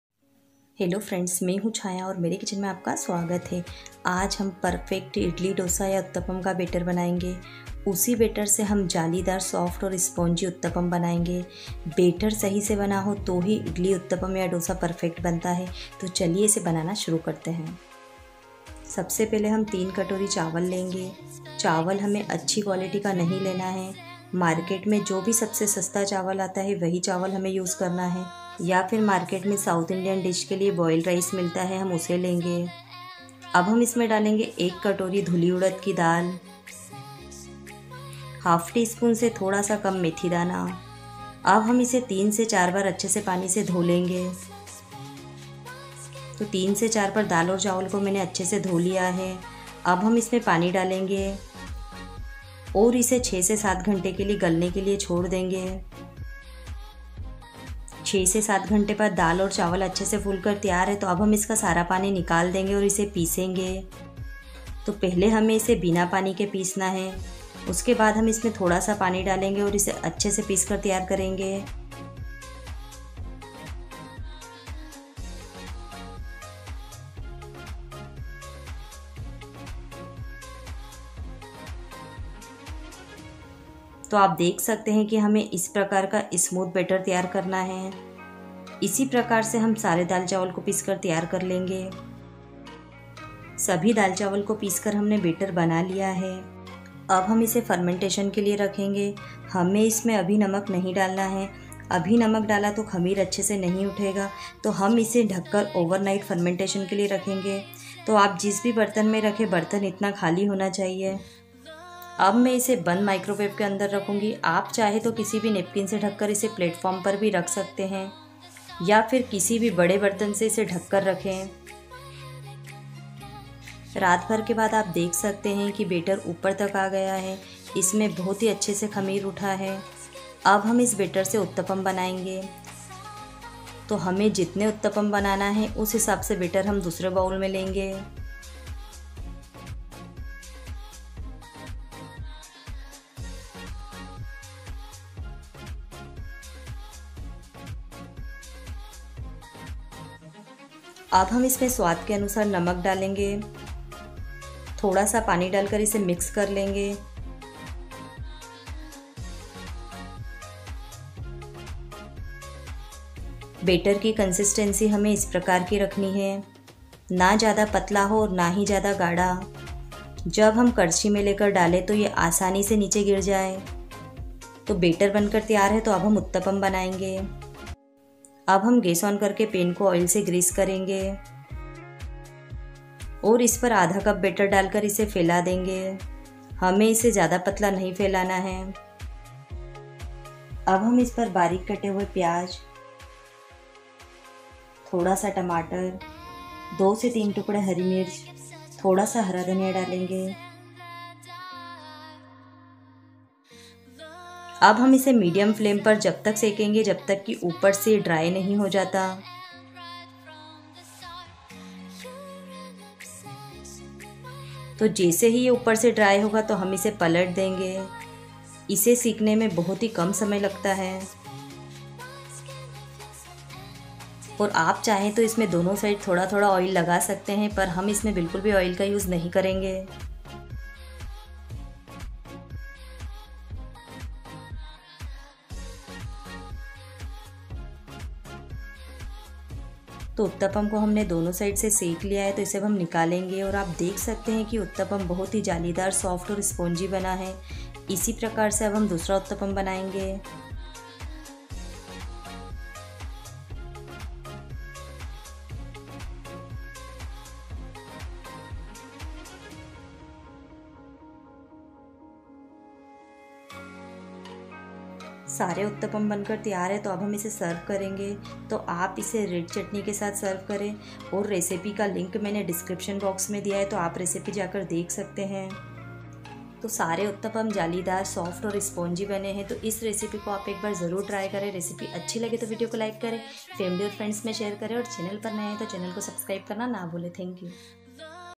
Perfect Idli,Dosa & Uttapam Batter/ Healthy Without Oil Sponge & Soft Uttapam Recipe
southindian 5minuteschutney South Indian Red Chutney For Idli,Dosa,Uttapam & Vada